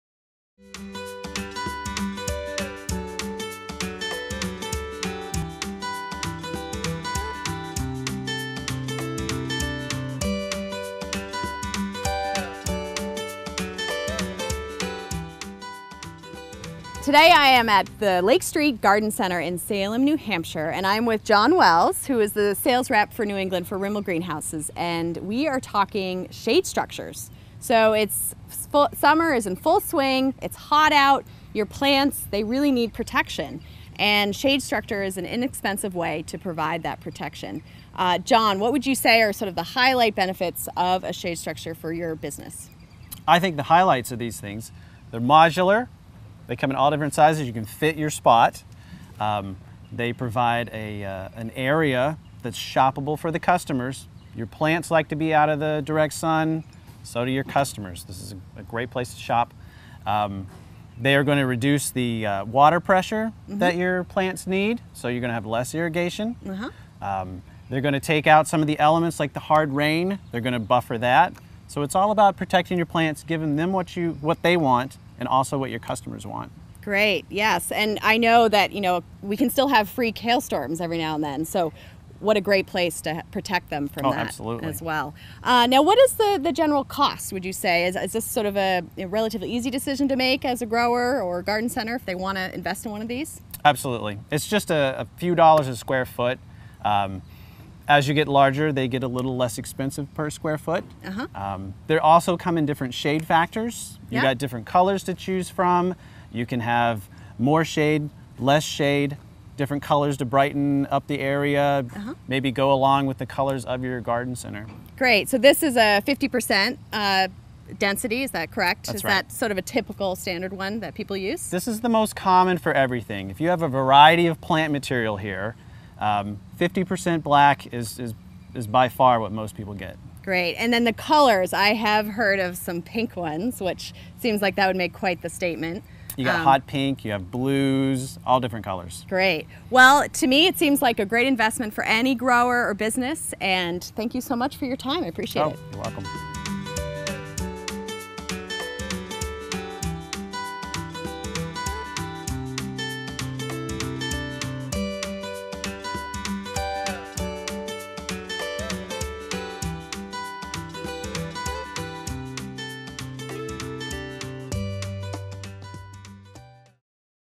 Shade Structures Part I: Why Shade Structures?
Clara Coleman is joined by our own John Wells to explain the many cost-effective benefits of shade structures for retail garden centers. For plants, protection from direct sunlight and hard rain creates optimal growing conditions; and for customers, perusing your plants in the comfort of the shade makes for a pleasurable shopping experience. Plus, Rimol shade structures can be branded to your garden center in color and with various density levels depending on the plants you grow.